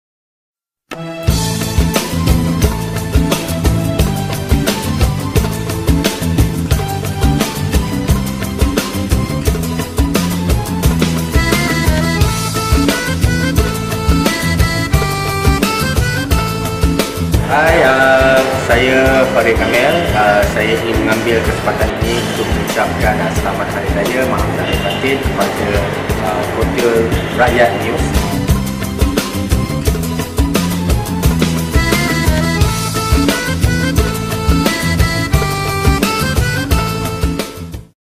Ucapan Salam Aidilfitri Farid Kamil
Ucapan raya oleh pelakon, Farid Kamil.